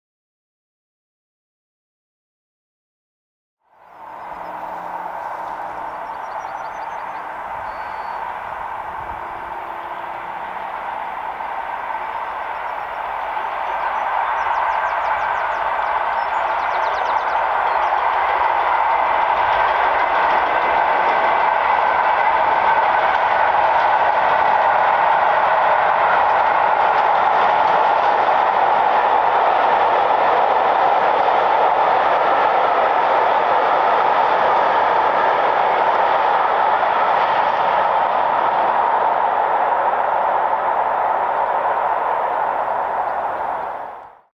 35028 Clan Line crossing Hurstbourne Viaduct
35028 Clan Line taking the End of Southern Steam Tour over Hurstbourne Viaduct on the way to Yeovil. 9/July 2022.